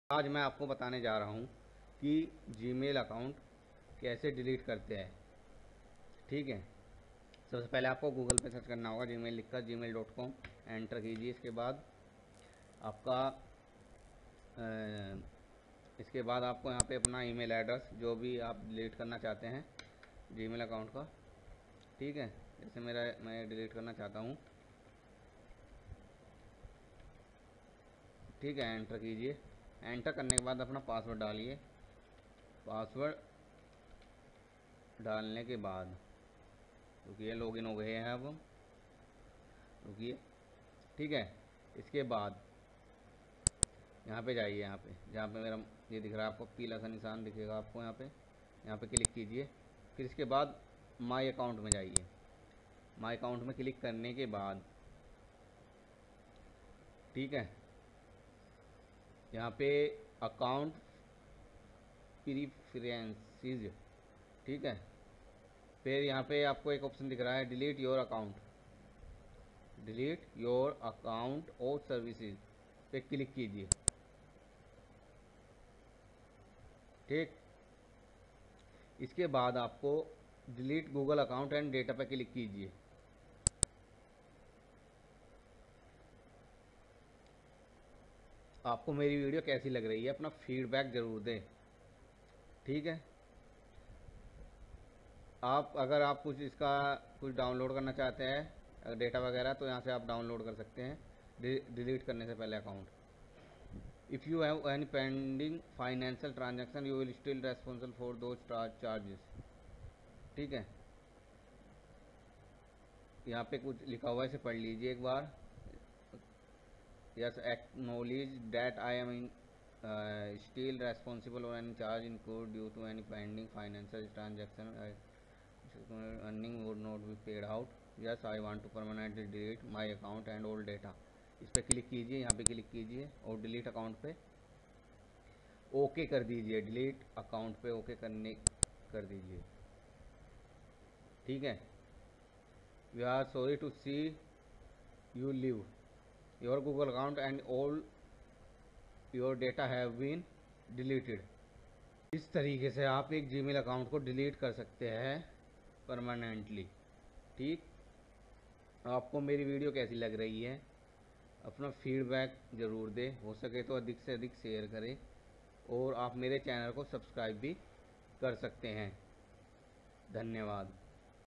How To Delete A Gmail Account Permanently
In this video, You will know. How To Delete A  Gmail Account Permannently step by step in hindi.